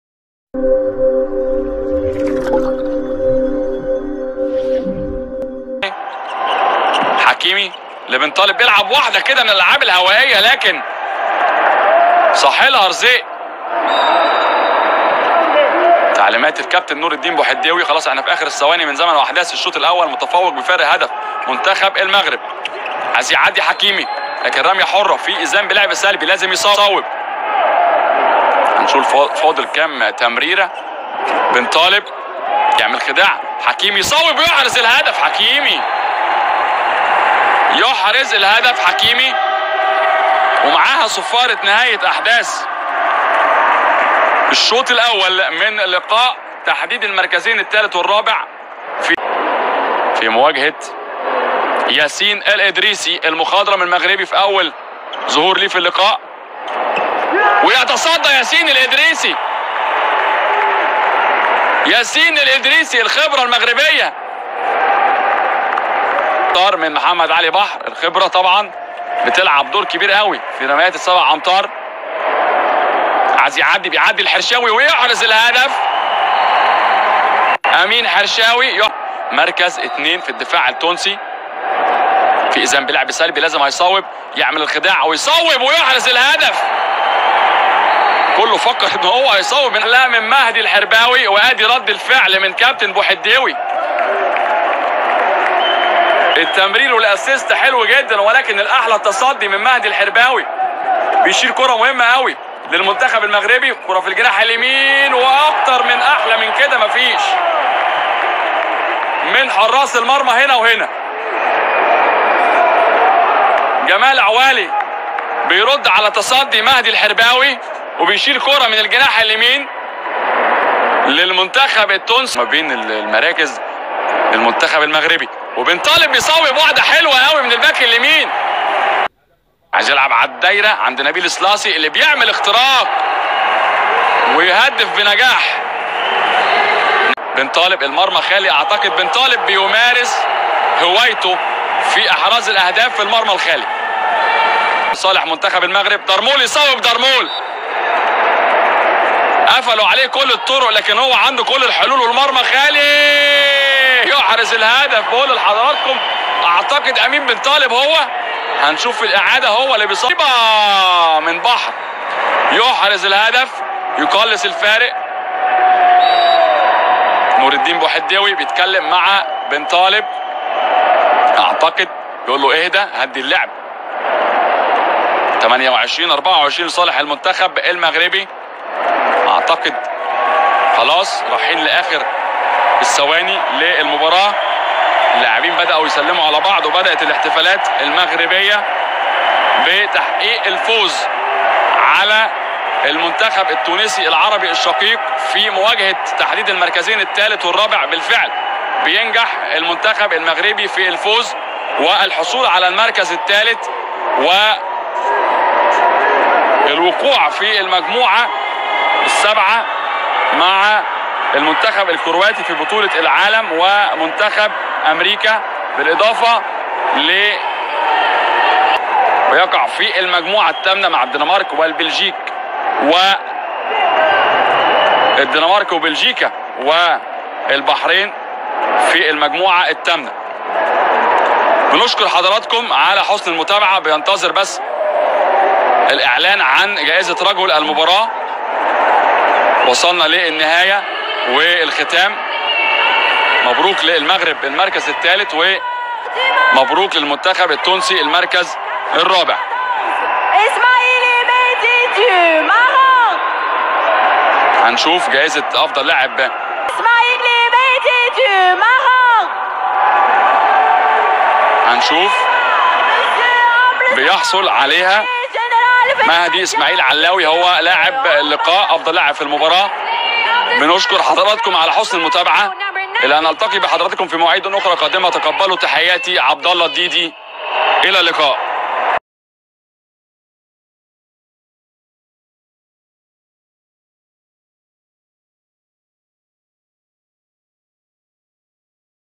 المنتخب المغربي لكرة اليد يفوز بالنحاسية بطولة إفريقيامصر بعد انتصاره على منتخب تونسي بنتيجة 28-24
المنتخب الوطني المغربي لكرة اليد يتحصل على المركز الثالث والميدالية البرونزية 🥉 في النسخة رقم 25 من بطولة إفريقيا دورة مصر 2022 بعد الانتصار على تونس بنتيجة 28-24

فوز تاريخي لأسود الأطلس على منتخب نسور قرطاج الذي سيجد نفسه خارج البوديوم الإفريقي لأول مرة منذ أول مشاركة له في النسخة الأولى التي احتضنها سنة 1974.

 ثامن تأهل إلى المونديال و ثاني بوديوم للمغرب بعد مركز ثالث في دورة تونس 2006 ودائما بقيادة الإطار نور الدين البوحديوي الذي في ظل كثير من العشوائية ينقذنا دائما وأعاد كرة اليد المغربية إلى سكة المشاركات المتتالية في المونديال بعد غياب عن 6 دورات سابقة.

شكرا للأبطال على القتالية و هنيئا لكم على عملكم طوال الشهر الماضي و بالتوفيق إن شاء الله في المونديال شهر يناير القادم و نجدد دعواتنا للنهوض بهذه الرياضة واهتمام واحتضان أفضل للمنتخبات الوطنية. لأننا نستحق و نسعى للألقاب والمنافسة على أعلى مستوى.